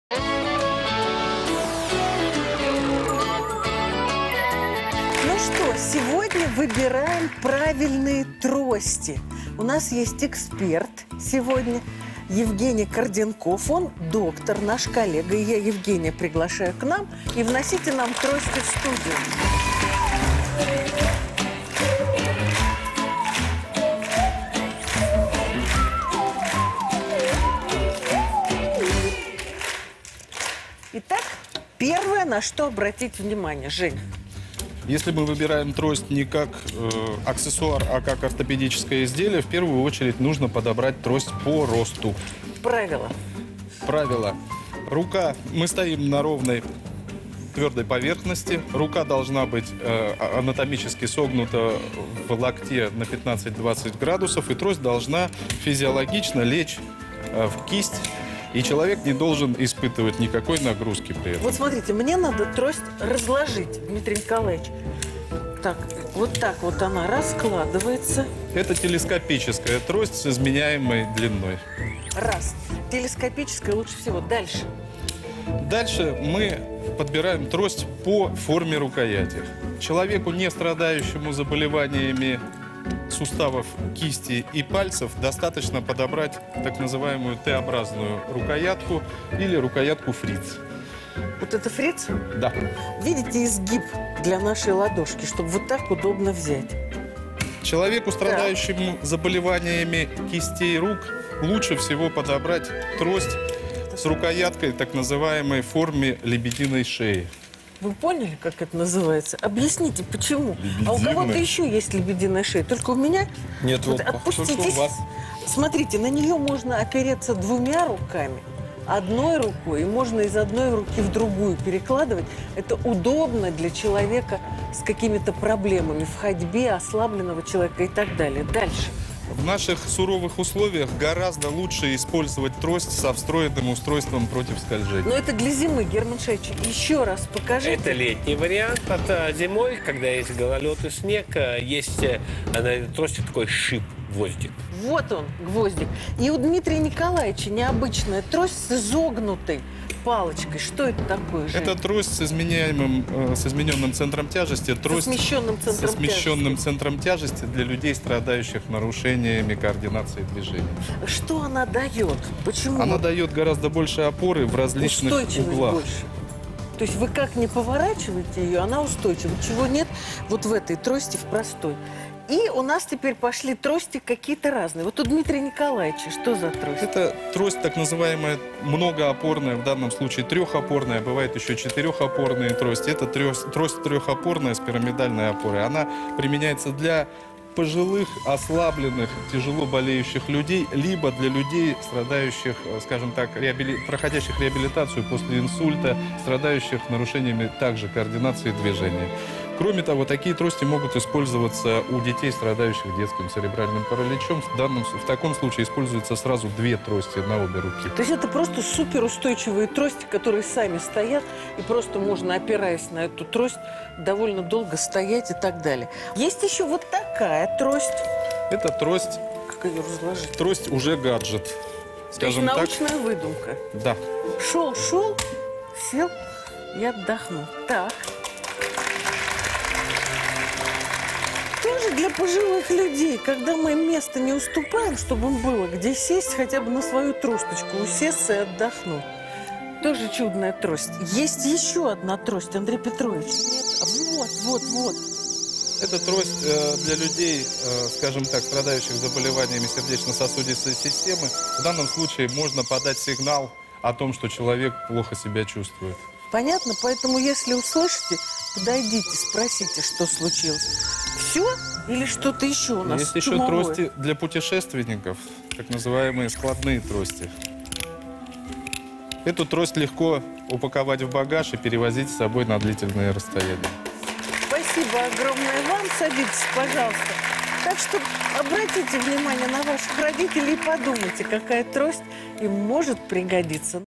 Как выбрать трость для пожилого человека. Медицинские рекомендации в передаче Елены Малышевой.
Елена Малышева рассказывает как правильно выбирать трость.

Первое, на что нужно обратить внимание - трость должна быть по росту.
Правило - на ровной твердой поверхности рука должна быть анатомически согнута в локте на 15-20 градусов и трость должна физиологично лечь в кисть и человек не должен
испытывать никакой нагрузки при этом.

Далее, мы подбираем трость по форме рукоятей. Человеку не страдающему заболеваниями суставов, кисти и пальцев, достаточно подобрать, так называемую, Т-образную рукоятку или
рукоятку фриц. Человеку, страдающему заболеваниями кистей рук лучше всего подобрать трость с рукояткой в форме лебединой шеи.

Гораздо лучше использовать трость со встроенным устройством против скольжения.